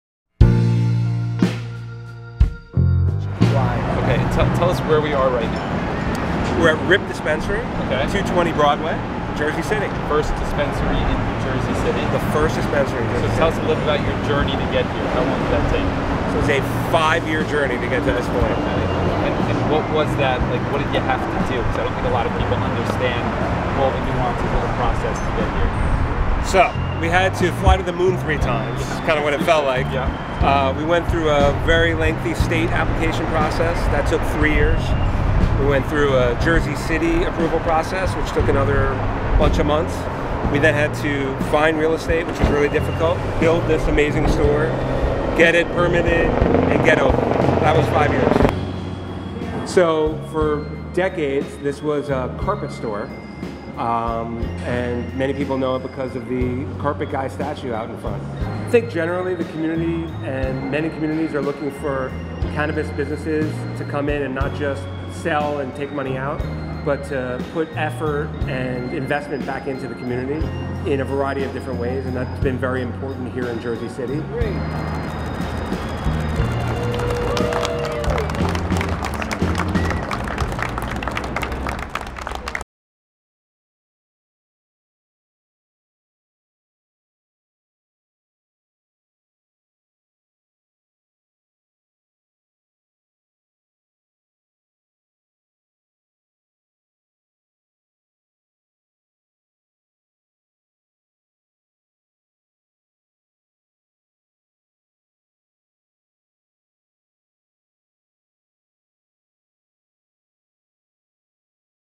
Jersey City Officially Cuts Ribbon on RIPT Dispensary
The Mayor recently welcomed Jersey City’s first medicinal cannabis dispensary. After years of paperwork and planning, RIPT was able to open at 220 Broadway.